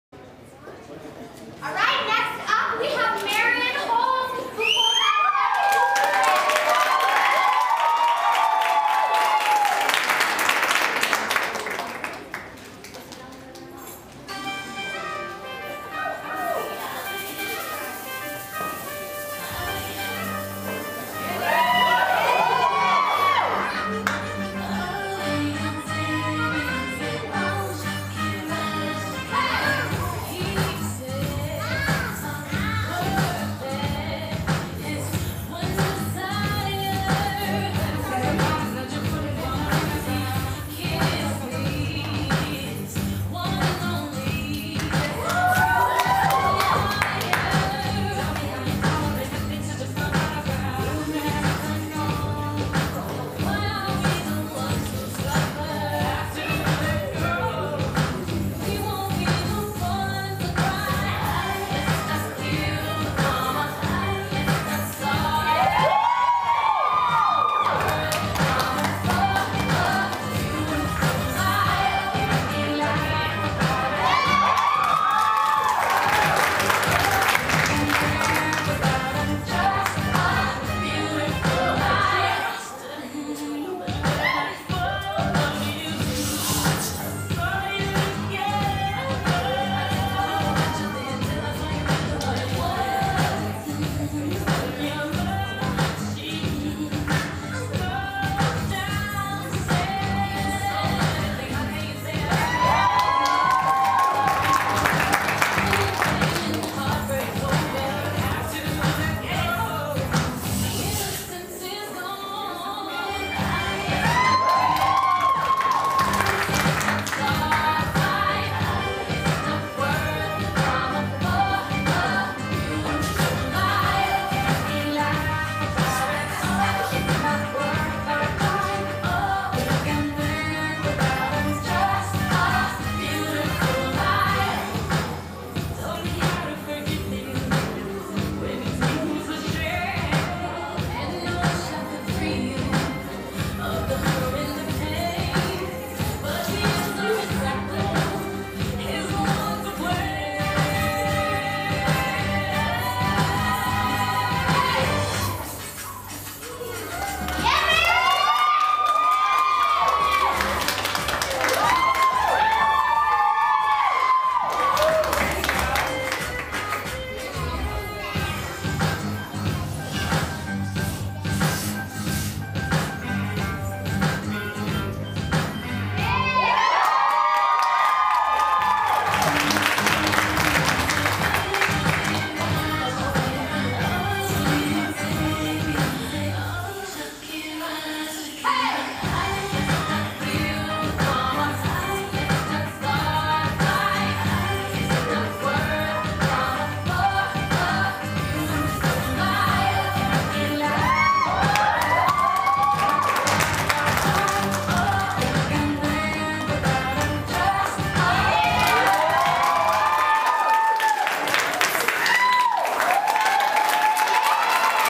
Marion's winning performance in the Intermediate Category at 2014 Epic Pole Competition and Showcase
Marion won intermediate category at the 2014 Epic Pole Competition and Showcase